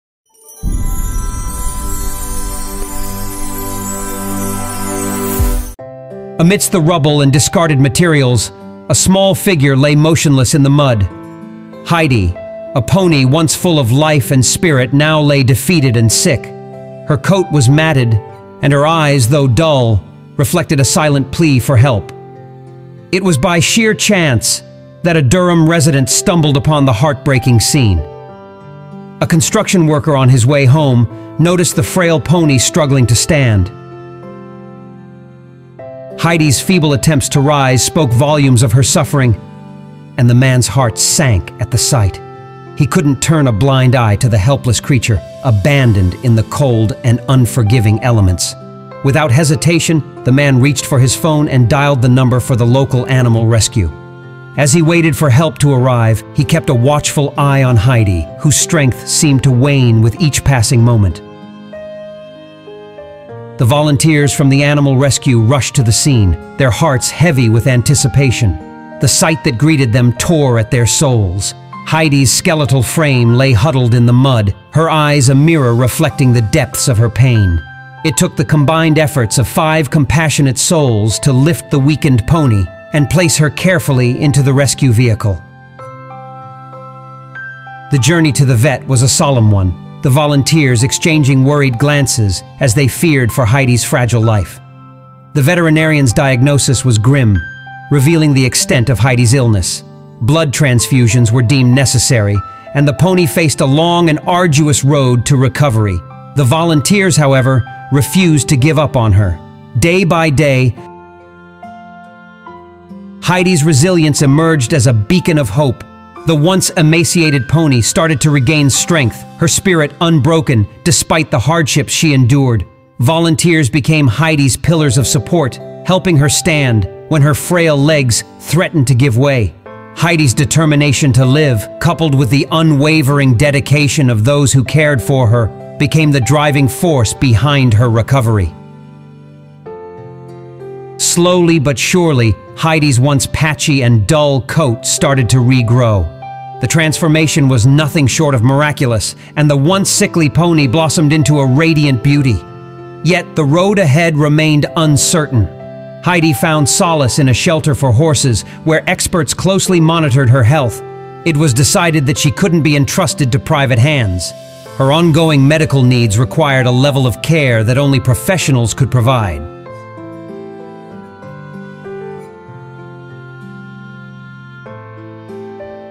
The Owners Left the Pony to Her Own Devices She Could No Longer Stand On Her Feet from Helplessness
Amidst the rubble and discarded materials, a small figure lay motionless in the mud.